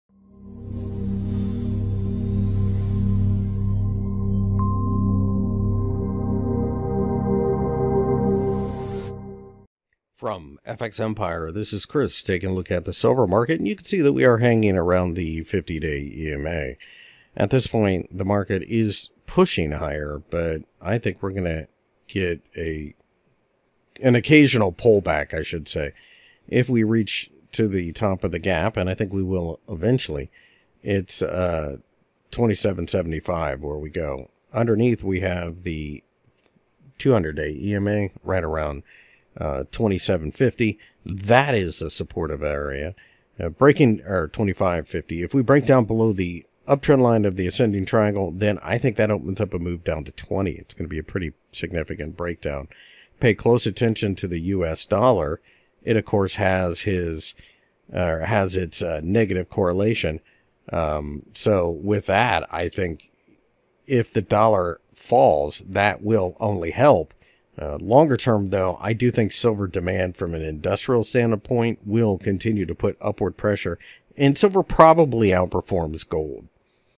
Silver Technical Analysis for July 06, 2021 by FXEmpire
The silver markets have rallied ever so slightly during the trading session on Monday but failed and the 50 day EMA again.